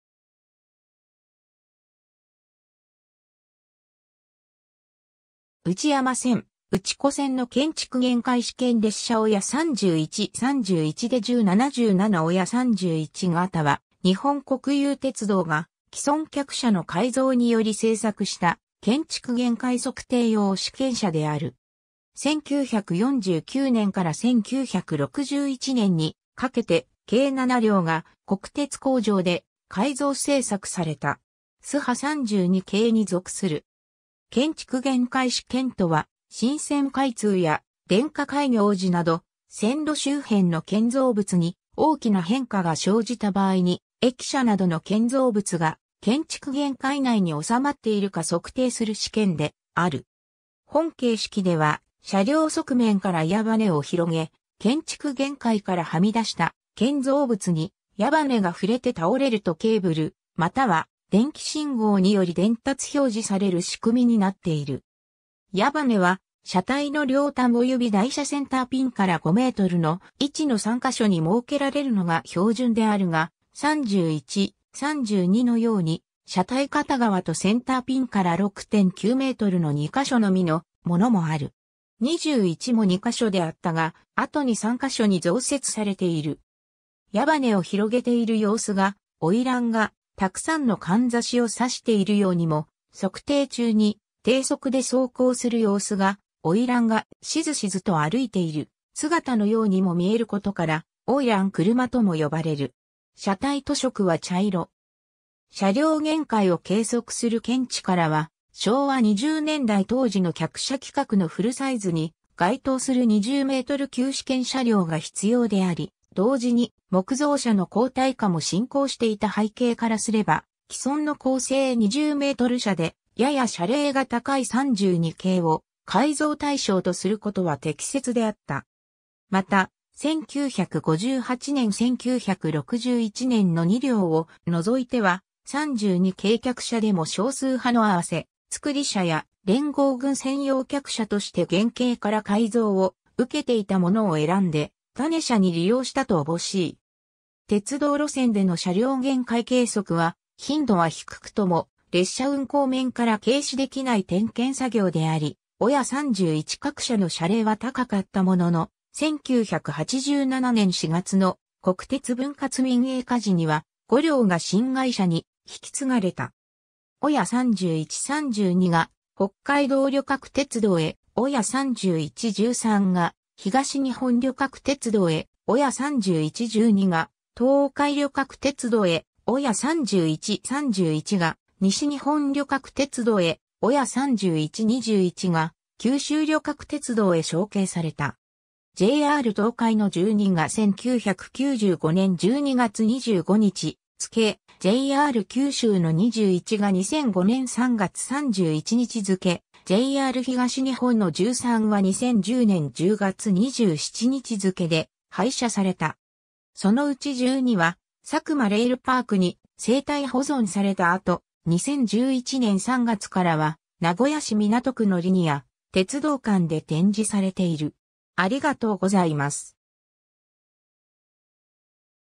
国鉄オヤ31形客車
内山線・内子線の建築限界試験列車オヤ31 31+DE10 77（1985年・現 新谷駅地点） オヤ31形は、日本国有鉄道（国鉄）が既存客車の改造により製作した建築限界測定用試験車（事業用客車）である。
1949年（昭和24年）から1961年（昭和36年）にかけて計7両が国鉄工場で改造製作された。
スハ32系に属する。
 建築限界試験とは、新線開通や電化開業時など、線路周辺の建造物に大きな変化が生じた場合に、駅舎などの建造物が建築限界（線路を走行する車両に障害物が触れないようにするため、周囲の建造物が越えてはならない限界線）内に収まっているか測定する試験である。
 本形式では車両側面から矢羽根を広げ、建築限界からはみ出した建造物に矢羽根が触れて倒れるとケーブルまたは電気信号により伝達表示される仕組みになっている。
矢羽根は、車体の両端及び台車センターピンから5mの位置の3か所に設けられるのが標準であるが、31、32のように車体片側とセンターピンから6.9mの2か所のみのものもある。
21も2か所であったが、後に3か所に増設されている。
 矢羽根を広げている様子が花魁（おいらん）が沢山のかんざしを挿しているようにも、測定中に低速で走行する様子が花魁がしずしずと歩いている姿のようにも見えることから「おいらん車」（あるいはカタカナ書きで「オイラン車」）とも呼ばれる。
車体塗色は茶色（ぶどう色2号）。
 車両限界を計測する見地からは、昭和20年代当時の客車規格のフルサイズに該当する20m級試験車両が必要であり、同時に木造車の鋼体化も進行していた背景からすれば、既存の鋼製20m車でやや車齢が高い32系を改造対象とすることは適切であった。
また、1958年・1961年の2両をのぞいては、32系客車でも少数派の合造車や連合軍専用客車として原型から改造を受けていたもの（一般営業向けの復元工事にも手数の掛かるもの）を選んで種車に利用したとおぼしい。
 鉄道路線での車両限界計測は頻度は低くとも列車運行面から軽視できない点検作業であり、オヤ31各車の車齢は高かったものの、1987年（昭和62年）4月の国鉄分割民営化時には、5両が新会社に引き継がれた。
オヤ31 32が北海道旅客鉄道（JR北海道）へ、オヤ31 13が東日本旅客鉄道（JR東日本）へ、オヤ31 12が東海旅客鉄道（JR東海）へ、オヤ31 31が西日本旅客鉄道（JR西日本）へ、オヤ31 21が九州旅客鉄道（JR九州）へ承継された。
 JR東海の12が1995年12月25日付け、JR九州の21が2005年3月31日付け、JR東日本の13は2010年10月27日付けで廃車された。
そのうち12は、佐久間レールパークに静態保存された後、2011年3月からは、名古屋市港区のリニア・鉄道館で展示されている。